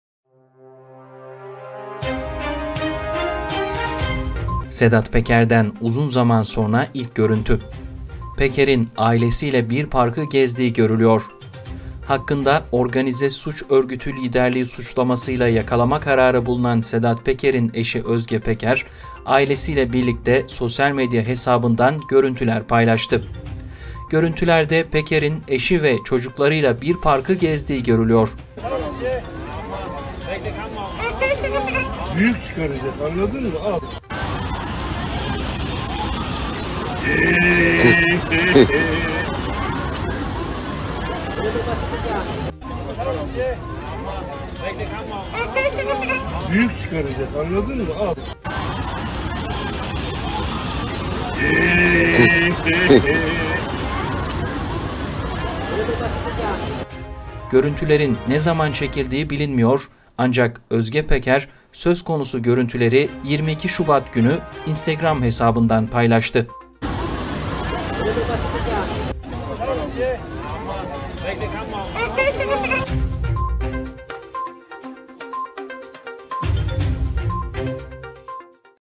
Sedat Peker’den Uzun Zaman Sonra İlk Video!
Sedat Peker’den uzun zaman sonra ilk video!
Peker’in ailesiyle bir parkı gezdiği görülüyor

Görüntülerde Peker’in eşi ve çocuklarıyla bir parkı gezdiği görülüyor.

Görüntülerin ne zaman çekildiği bilinmiyor ancak Özge Peker söz konusu görüntüleri 22 Şubat günü Instagram hesabından paylaştı.

haber özel,
haber kanalları,
haber aç ,
haber son dakika
son dakika
elektrik faturaları
elektrik faturası son dakika
doğal gaz